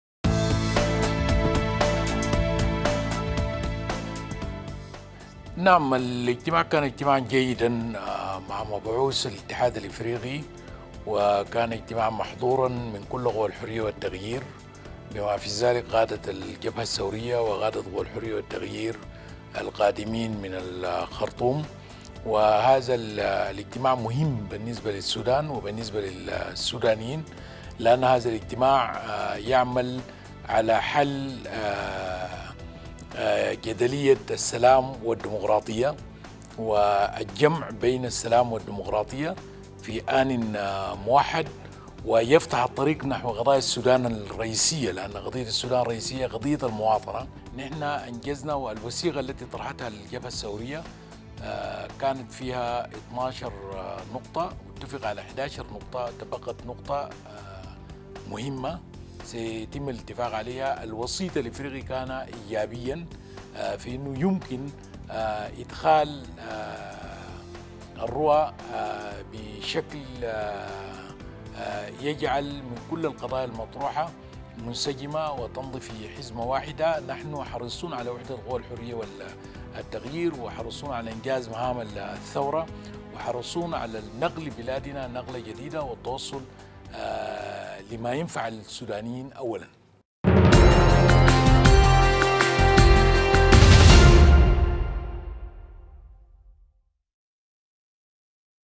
ياسر عرمان: حريصون على وحدة قوى الحرية والتغيير
العين الإخبارية | ياسر عرمان: حريصون على وحدة قوى الحرية والتغيير
تدور مفاوضات في العاصمة الإثيوبية أديس أبابا بين وفد من "قوى الحرية والتغيير" السودانية و"الجبهة الثورية"، وهو تحالف يضم أهم الحركات المسلحة.